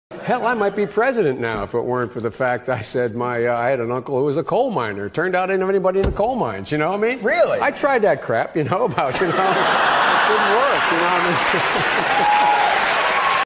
Joe Biden lied about his family being coal miners
16 years ago today, Joe Biden claimed he would have been president if he hadn’t LIED about his family being coal miners.

“I tried that crap… it didn’t work!”